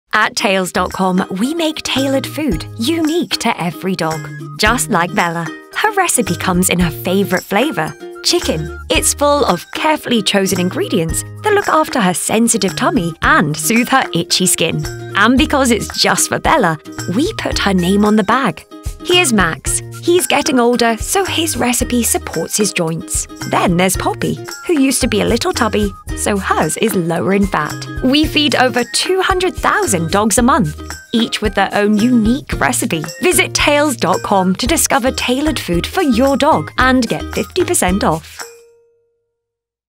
How does tails.com work? Introducing tailored nutrition
Here's how we create a unique recipe for every dog - perfectly portioned and in the taste they love, to keep them healthy and happy for life